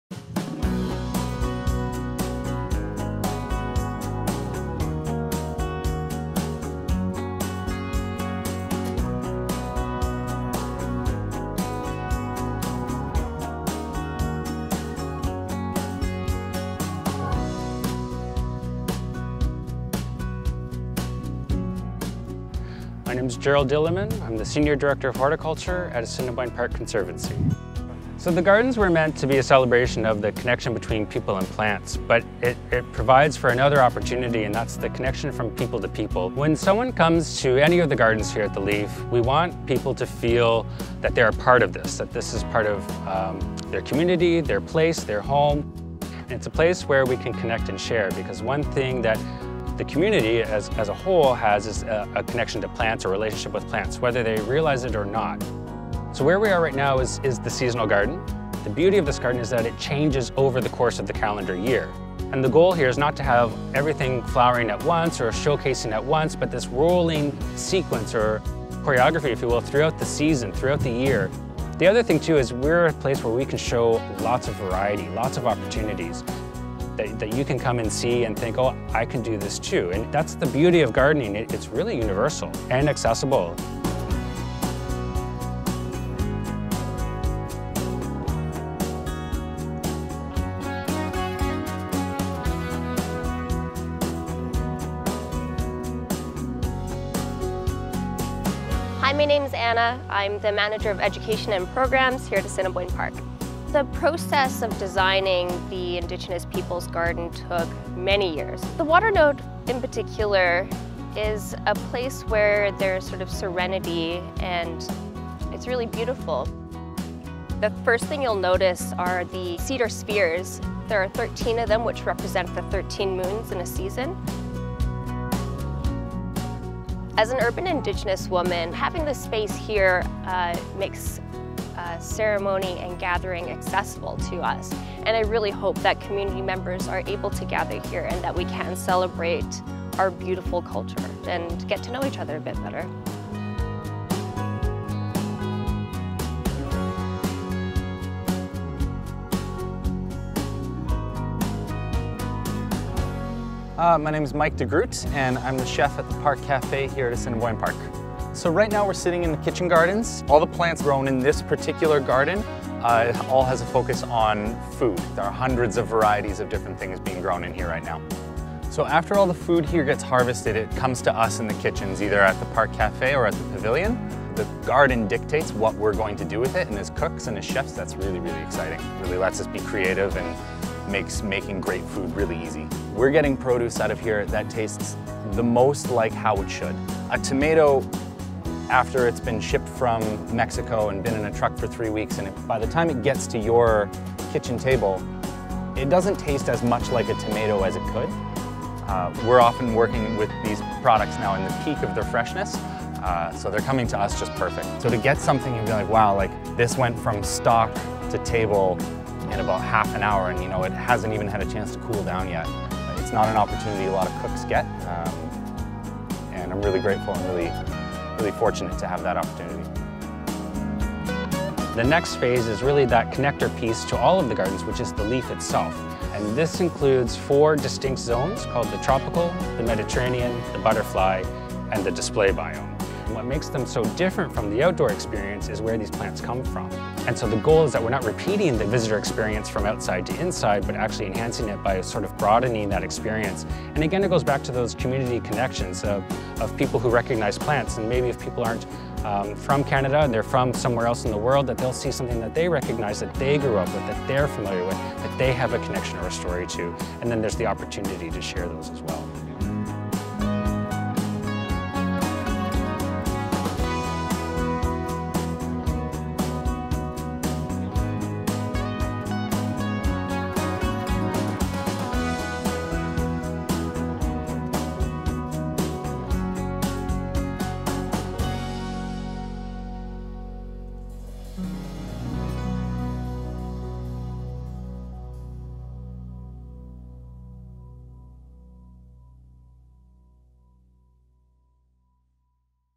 Gardens at the Leaf: Celebrating connections between people, plants & community
Discover community connections by taking a tour of Assiniboine Park Conservancy's Gardens at the Leaf with special focus on the Seasonal, Kitchen and Indigenous Peoples Gardens.

A visit to Manitoba means travelling through Treaty 1, 2, 3, 4 and 5 Territory and through communities who are signatories to Treaties 6 and 10. It encompasses the original lands of the Anishinaabeg, Anish-Ininiwak, Dakota, Dene, Ininiwak and Nehethowuk and the homeland of the Métis.